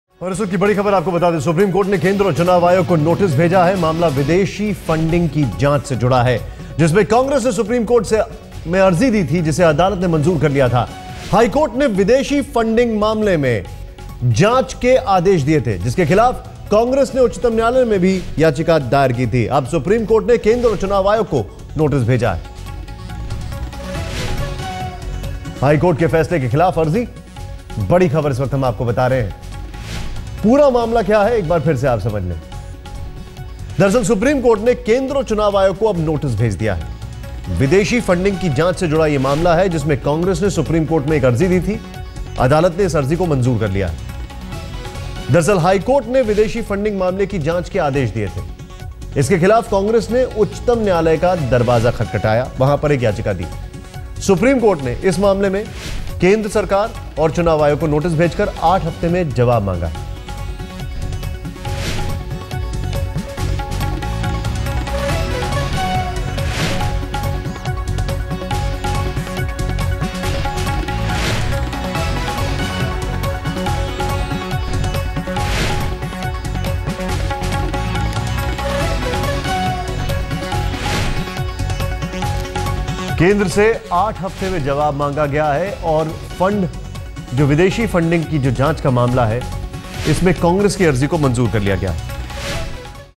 Supreme Court notices to Centre, EC on foreign funding case
The Supreme Court has issued notices to the Centre and the Election Commission on a petition challenging a Delhi High Court direction to investigate the Congress party's foreign funding.